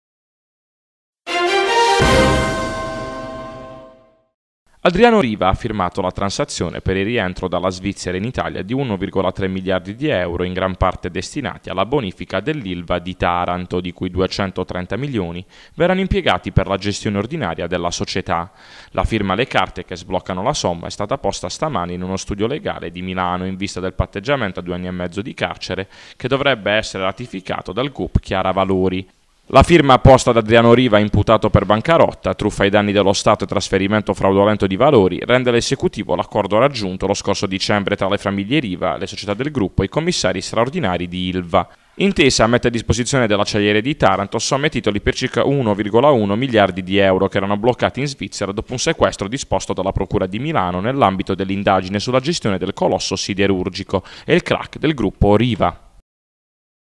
TARANTO | Ilva, firmata transazione: rientrano 1,3 milardi di euro
Due anni e mezzo di carcere per Adriano Riva